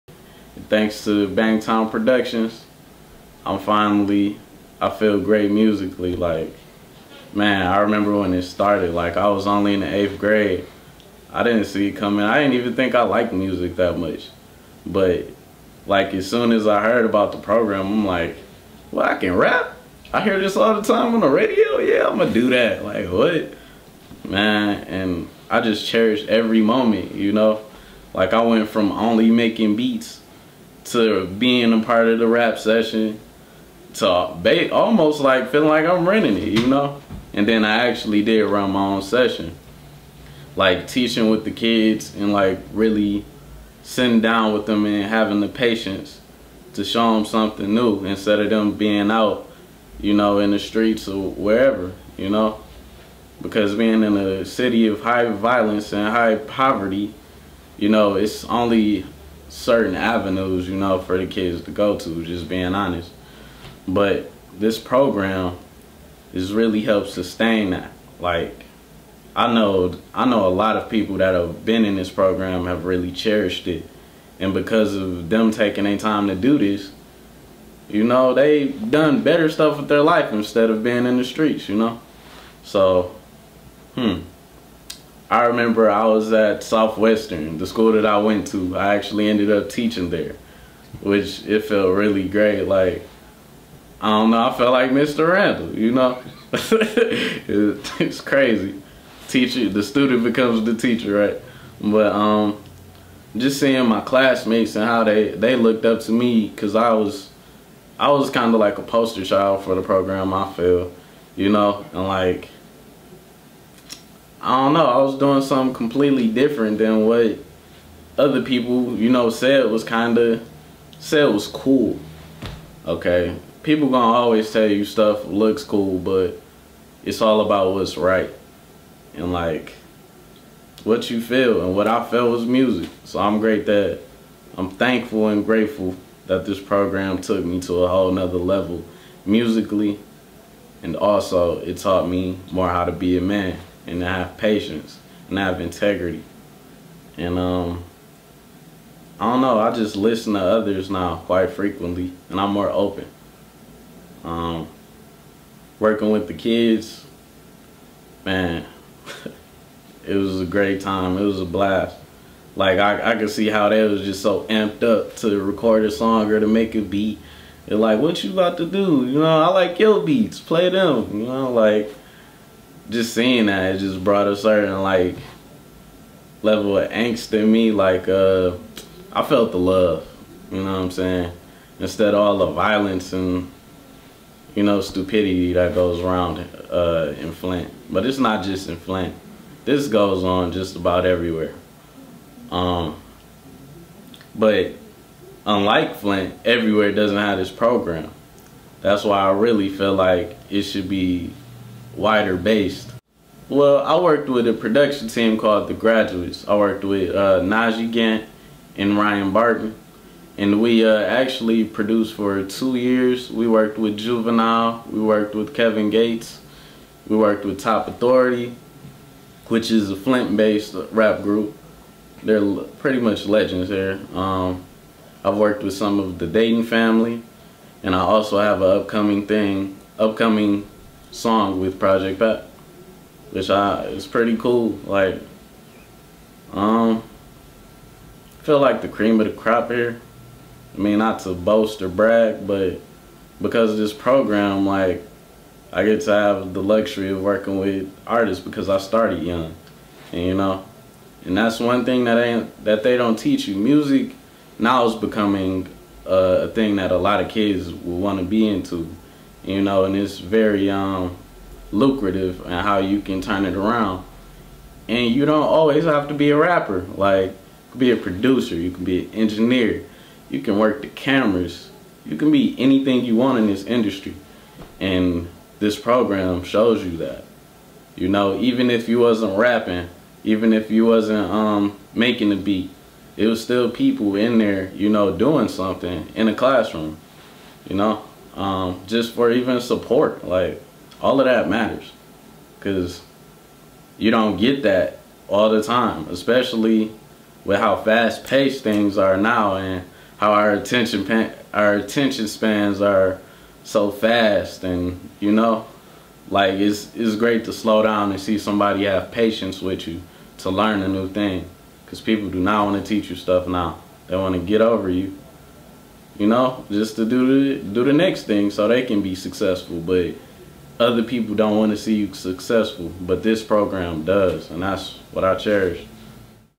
Bangtown SOTG Prodby DET
Willie Smith Jr. aka Prodby DET talks about his experience with Bangtown Productions & Recordings - Studio On The GO as a student, teacher and his life now as a producer.